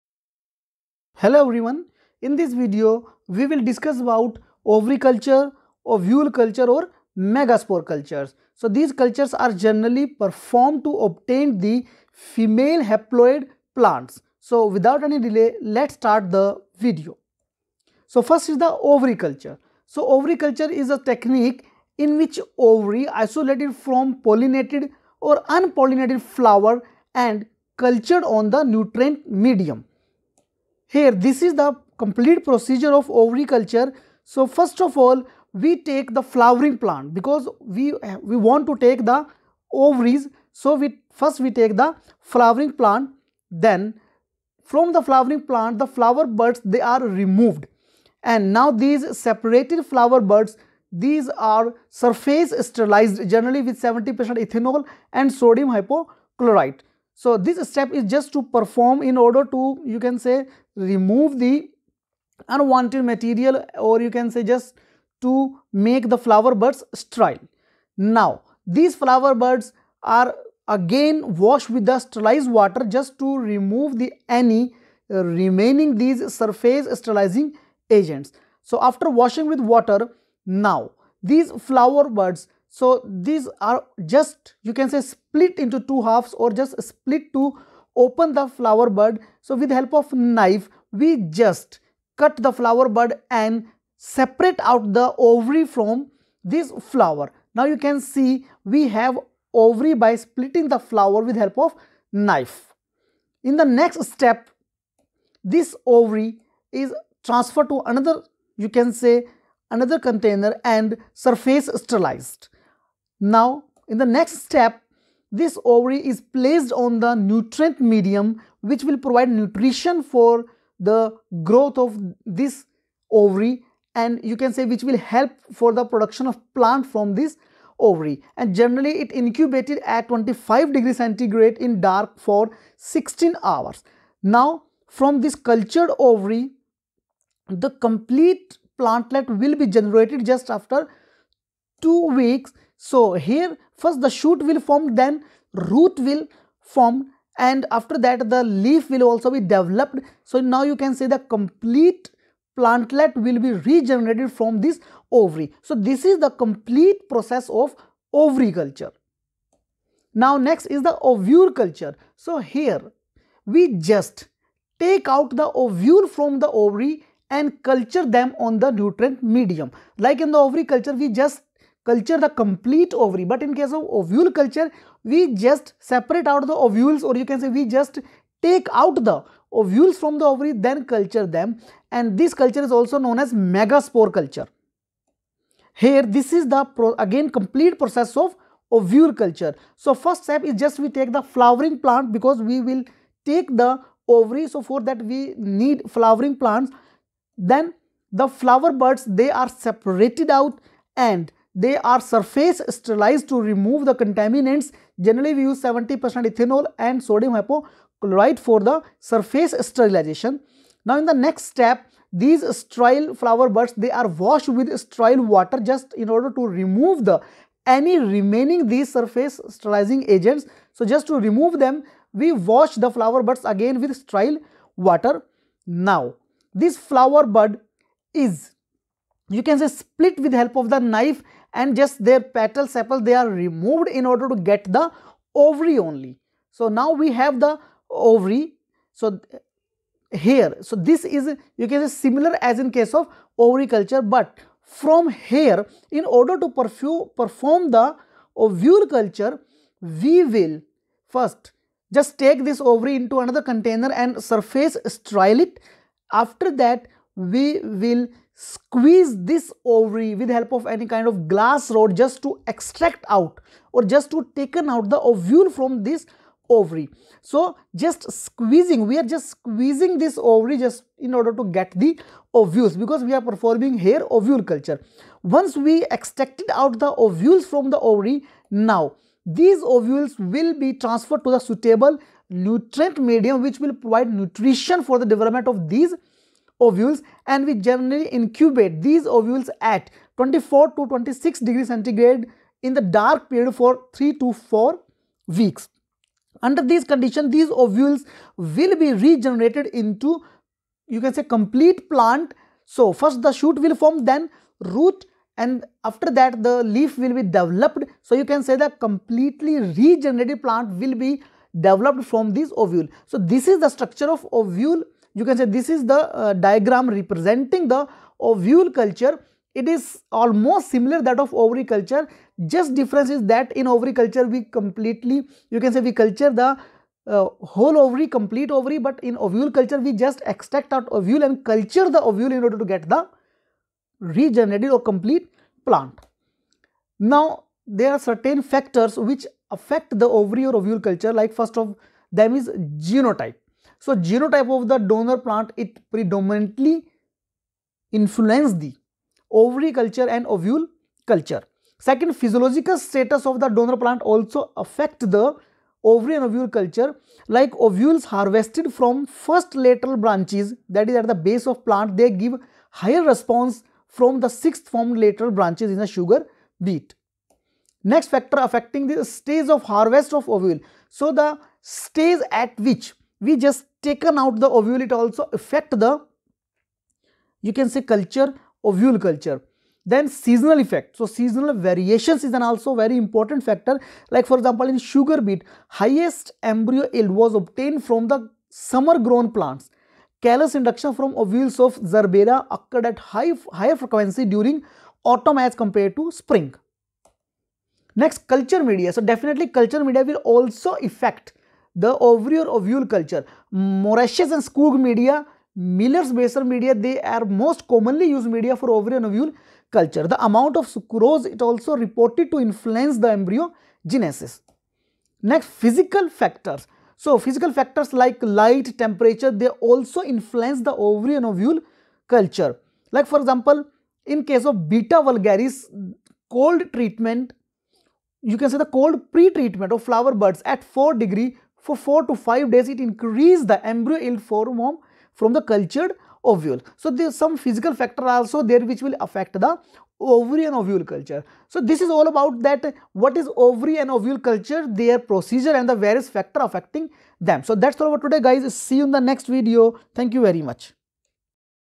Ovary and Ovule or Megaspore culture | Production of female haploid plants | Gynogenesis
In this video you will learn ovary and ovule or megaspore culture in plants. These cultures are used for production of female haploid plants.